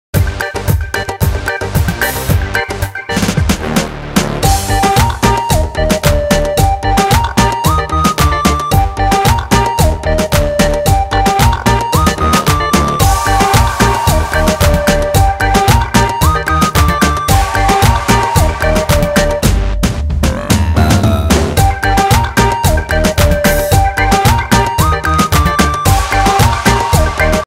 *YELLO TERRAPIN TURTLE* Caught on fishing Rod - Sump Skildpadde fra naturen i Danmark
Medefiskeri - Under et aften fisketur i skalle søen, dukkede denne flotte guløret sumpskildpadde op fra det dybe vand, fanget på en simpel fisket bund fiskestang med et enkelt MAJSKORN og en 12`er krog.